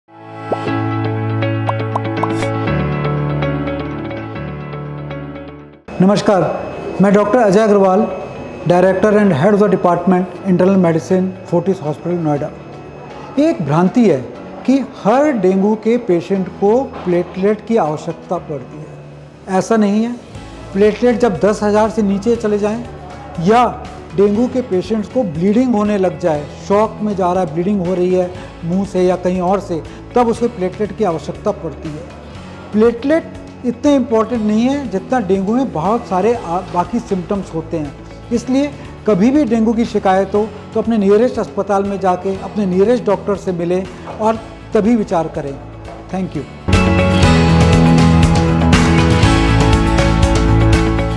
Dr. Ajay Agarwal debunks the myths related to the Dengue
It is a common misconception that all dengue cases require platelet transfusions. However, this is not true. Platelet transfusions are not necessary for every individual with dengue fever. Unnecessary platelet transfusions can lead to potential risks and complications. Listen to Dr. Ajay Agarwal, Director, Internal Medicine, Fortis Hospital, Noida, as he debunks the myth related to the same.